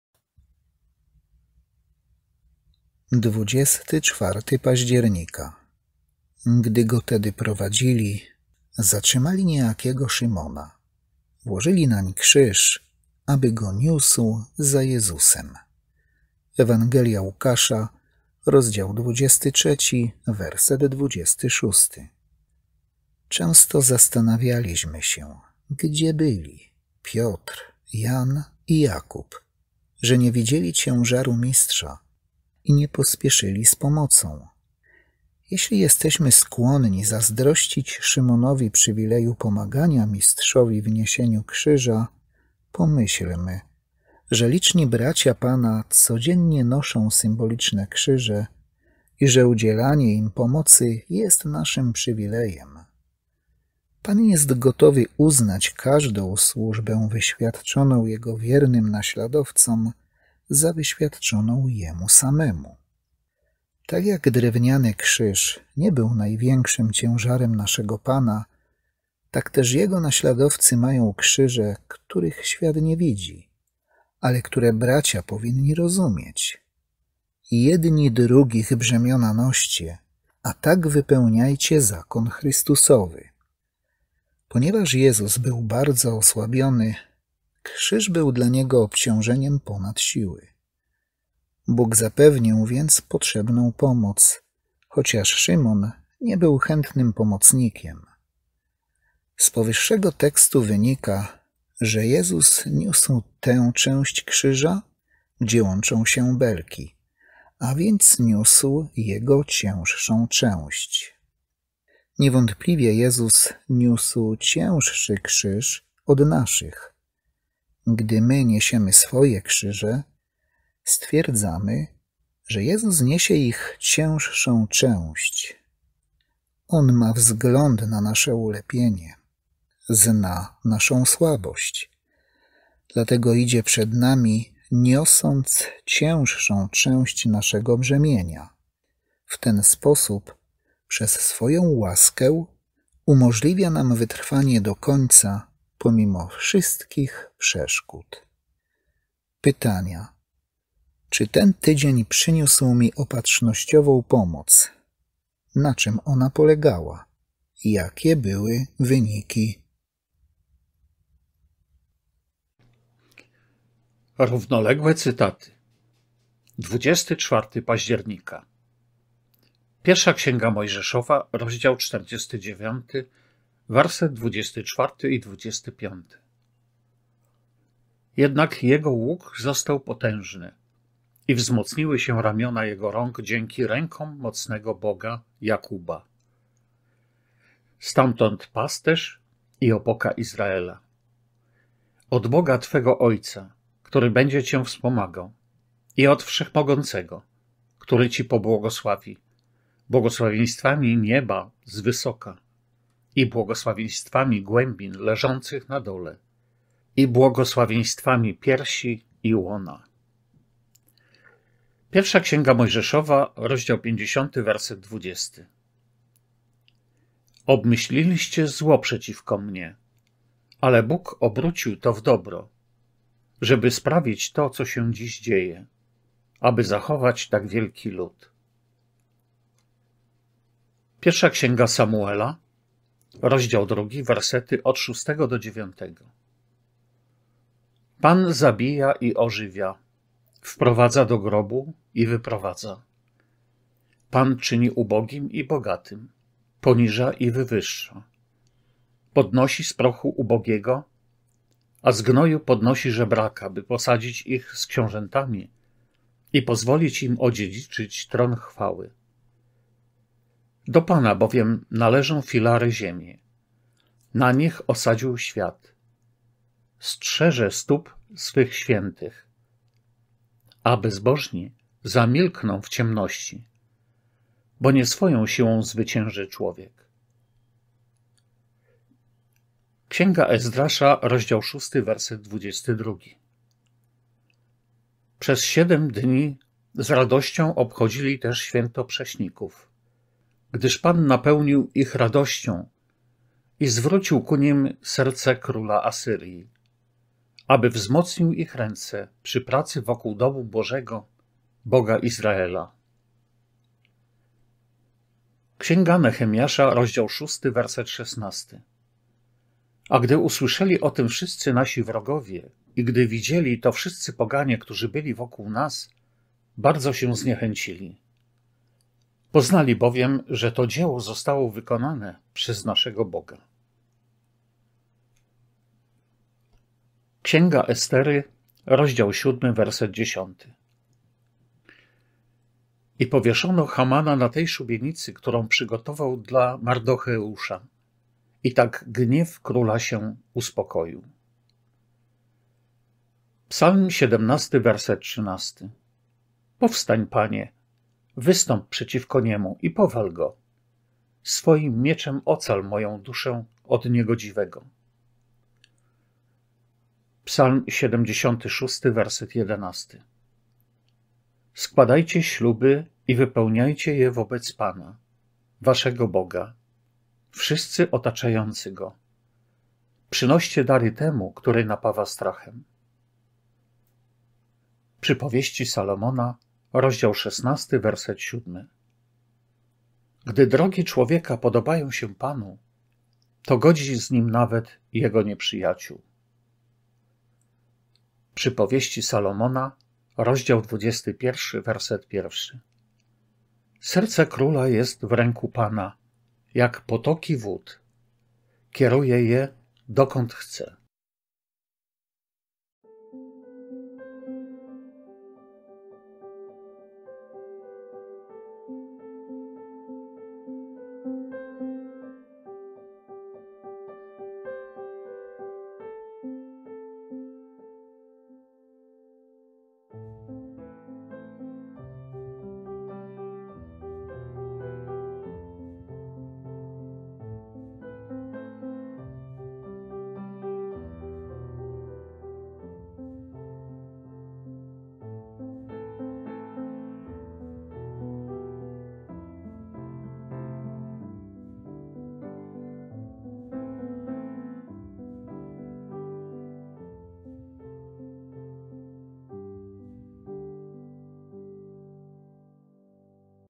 Manna 24 Październik
Gdy go tedy wiedli, uchwyciwszy Szymona ... włożyli nań krzyż, aby go niósł za Jezusem
Łuk. 23:26
Często zastanawialiśmy się, gdzie byli Piotr, Jan i Jakub, że nie widzieli ciężaru swego Mistrza i nie pośpieszyli z zaofiarowa- niem Jemu pomocy? Jeśli jesteśmy skłonni zazdrościć Szymono- wi przywileju udzielenia Mistrzowi pomocy w niesieniu krzyża, pomyślmy, że liczni Pańscy bracia codziennie noszą symboliczne krzyże i że udzielanie im pomocy jest naszym przywilejem, a tak- że, iż Pan zgodził się uznawać każdą posługę świadczoną Jego wiernym naśladowcom za świadczoną Jego własnej osobie. Tak jak drewniany krzyż nie był największym ciężarem dla naszego Pana, tak też Jego naśladowcy mają krzyże, których świat nie wi- dzi, ale które bracia powinni rozumieć. „Jedni drugich brzemiona noście, a tak wypełniajcie zakon Chrystusowy”.
Z powodu osłabienia krzyż wyczerpał siły Jezusa do granic wytrzy- małości, a wówczas Bóg przygotował potrzebną pomoc, chociaż Szymon był niechętnym pomocnikiem. Powyższy tekst daje do zrozumienia, że Je- zus niósł krzyż z tej strony, gdzie belki się łączą, stąd niósł jego cięższą część. Niewątpliwie Jezus niósł cięższy krzyż od naszych. A kiedy niesie- my swoje krzyże, stwierdzamy, że Jezus niesie ich cięższą część. On ma wzgląd na nasze „ulepienie”, zna naszą słabość, dlatego idzie przed nami, niosąc cięższą część naszego brzemienia. I tak przez Swoją łaskę, umożli- wia nam wytrwanie do końca, pomimo wszelkich przeszkód.
Równoległe cytaty: 1 Mojż. 49:24, 25; 50:20; 1 Sam. 2:6–9; Ezdr. 6:22;
Neh. 6:16; Est. 7:10; Ps. 17:13; 76:11; Przyp. 16:7; 21:1.
Pieśni: 121, 63, 99, 124, 129, 149, 252.
Pytania: Czy ten tydzień przyniósł mi opatrznościową pomoc? Jakie to były opatrzności? Jakie były wyniki?